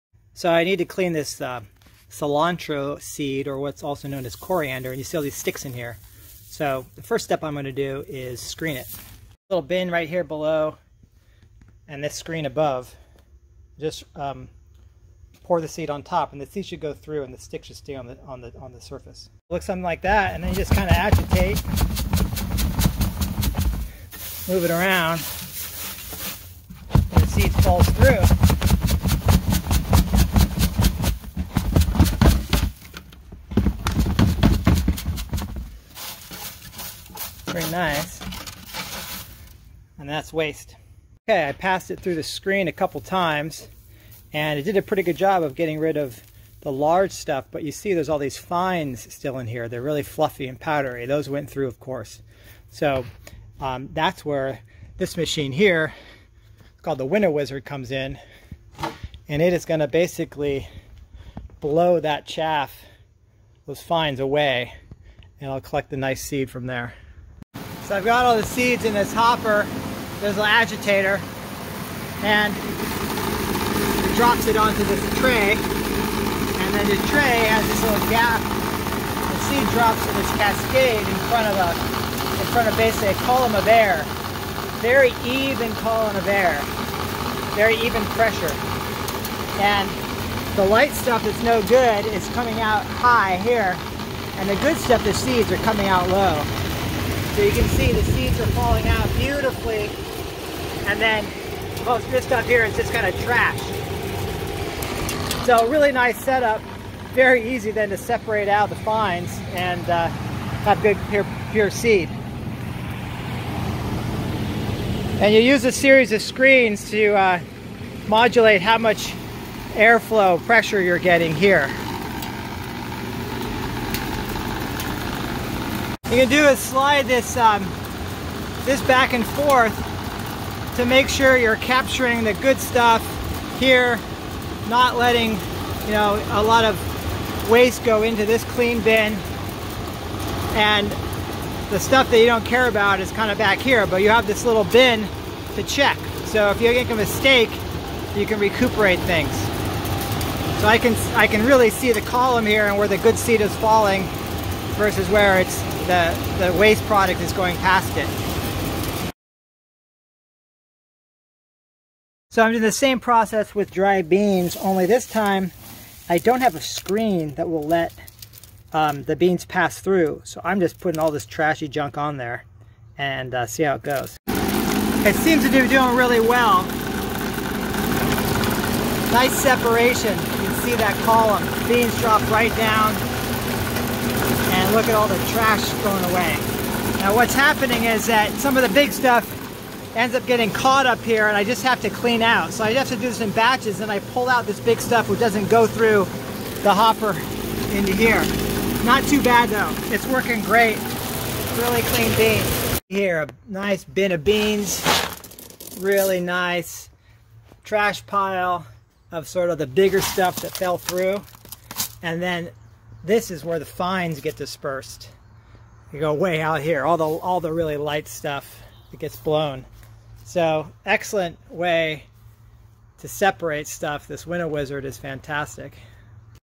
Seed Cleaning with Winnow Wizard
It truly is difficult to overstate the importance of seeds to human societies. So as a farmer who plants a lot of seeds I also am keen on harvesting my own. The complexity tends to come when you want to turn a harvest into more or less pure seed that can be replanted. I have some experience with this from being in the Willamette Valley and having some history with the very large seed cleaners in the area. But more recently I have been learning to use the famous Winnow Wizard developed here in Corvallis, Oregon and it is a technology very intuitive and especially useful for farmers at a modest scale, like myself.

Here's a quick primer on how this works. After harvest you probably need to thresh the bulky material that is stems, leaves, and parts of the mature fruits that enclose the seeds. This creates a mixture of materials with differential shapes and densities. Using screens that select for different sizes you can narrow down the bulk of what is left that includes seeds.

The Winnow Wizard works by creating a very consistent flow of air and then the material is dropped into this air column. Differences in density (aka specific gravity) come into play and usually the seeds drop faster than the lighter material in your mixture. Also, you could have material with the same density but different shapes and these could differ in how they are captured and moved by airflow.

The video shows me cleaning cilantro seed, which is also known as the spice coriander, as well as a dry pinto type bean known as Whipple. We have used this machine for many kinds of seeds, and I am a bit smitten by it.